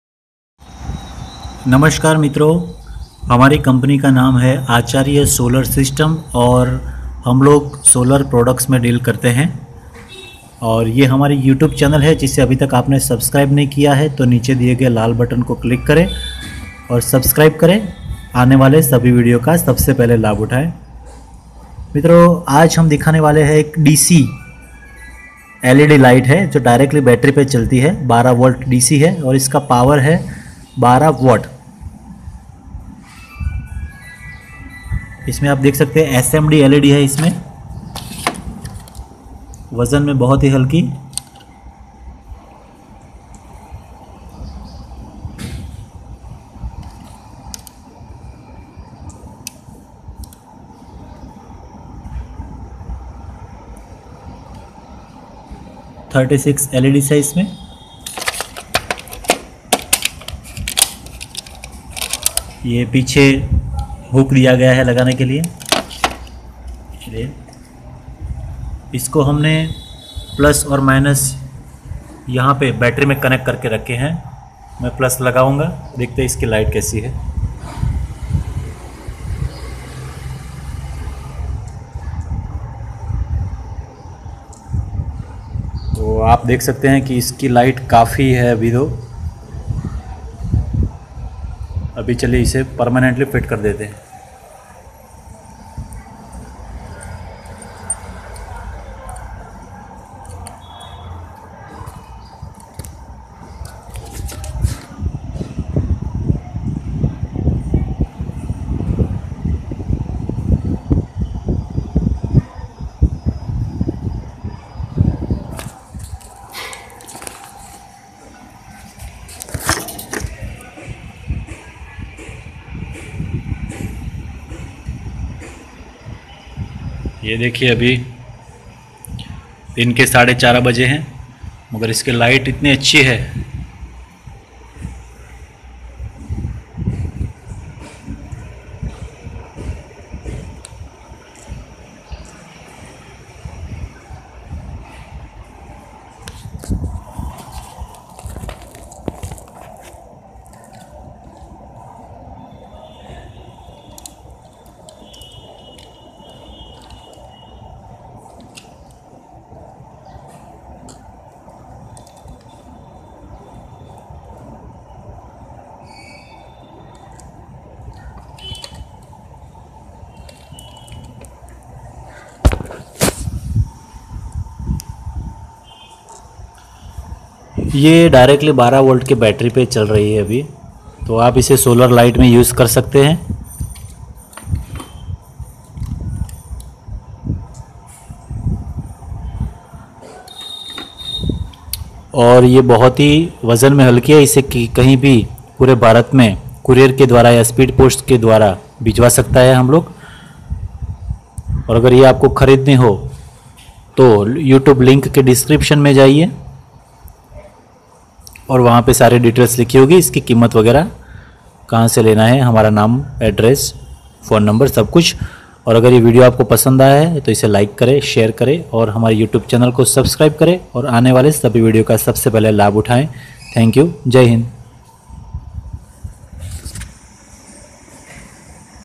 12 वोल्ट्स की बैटरी पे चलेगी ये लाइट सिर्फ 250/-
Led light work directly on 12 volt battery

Price 250/-

Marketed by ACHARYA SOLAR SYSTEM GUJARAT MO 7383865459

Inquiry time Monday to Saturday between 10:30 AM to 7:30 PM

Sunday holiday